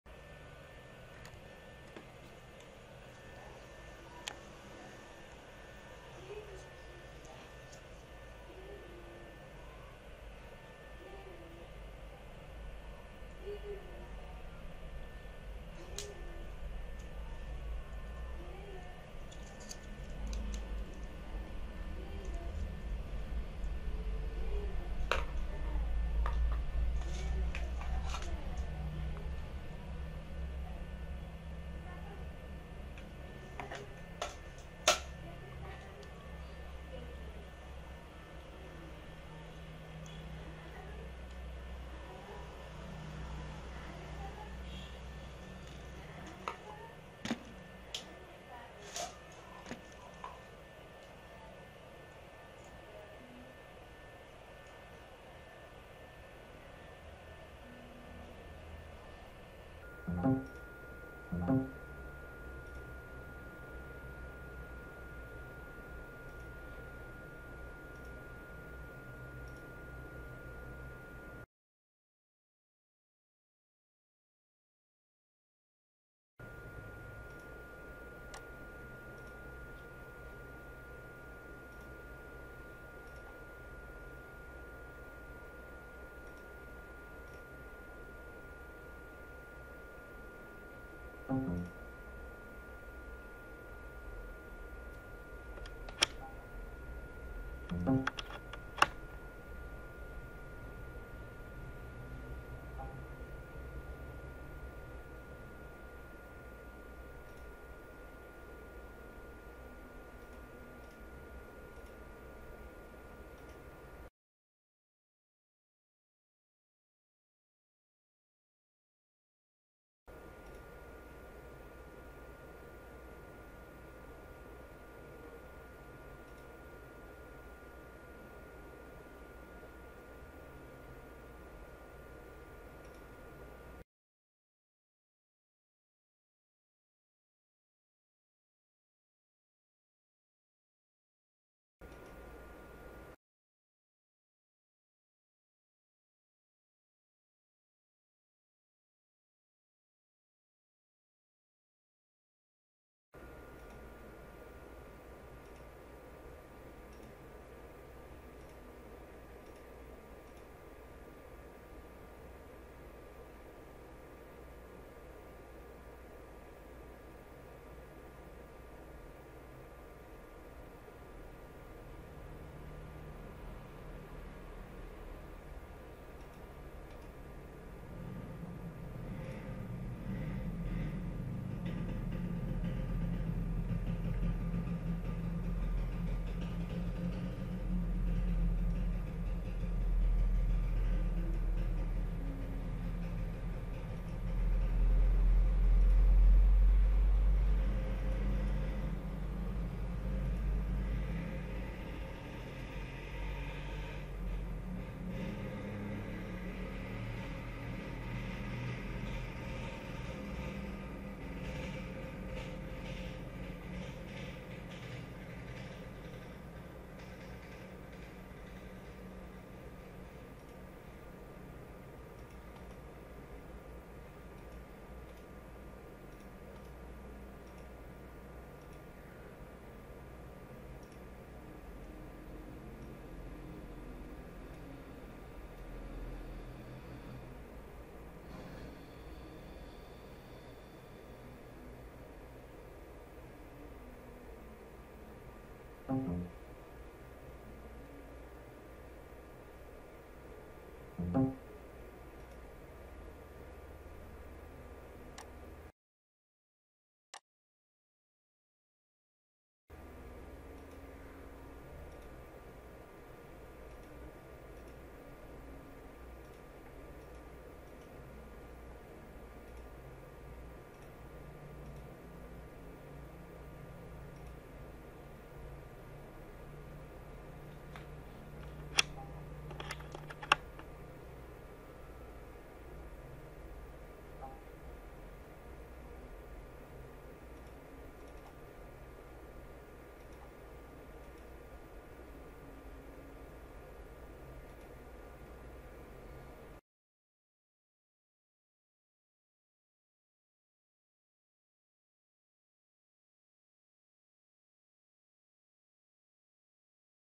IDE/ATA SATA to USB Cable Converter
This is guide how to use IDE/ATA SATA to USB Cable Converter